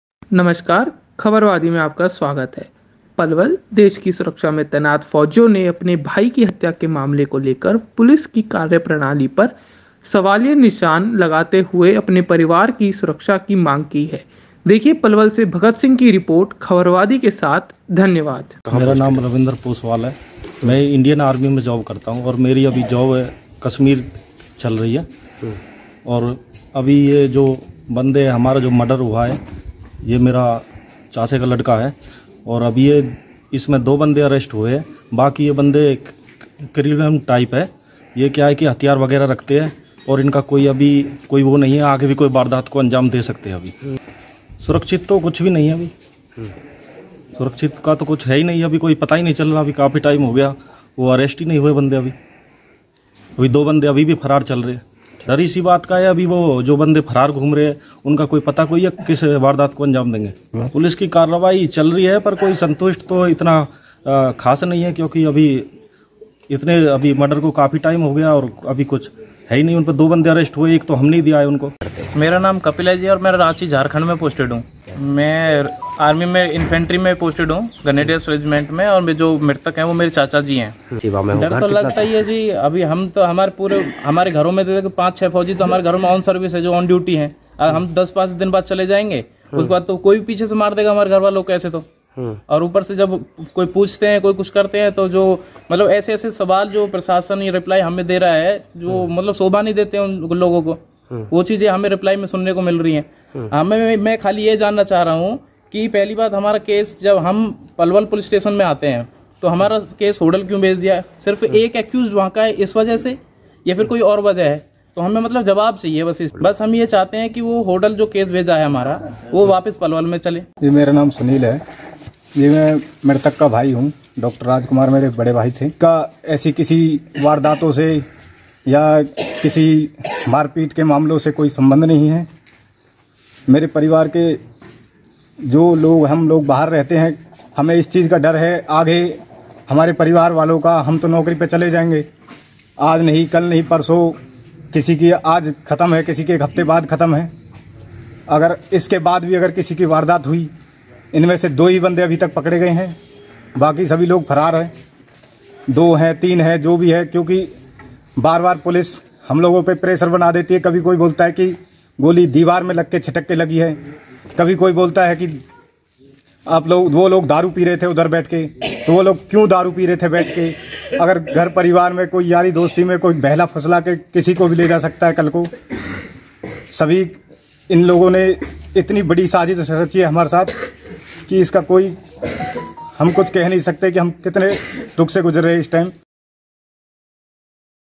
पीड़ित परिवार ने लगाई पुलिस से सुरक्षा की गुहार, कहा हत्यारोपी उनके परिवार पर कर सकते हैं हमला
पलवल से भगत सिंह की रिपोर्ट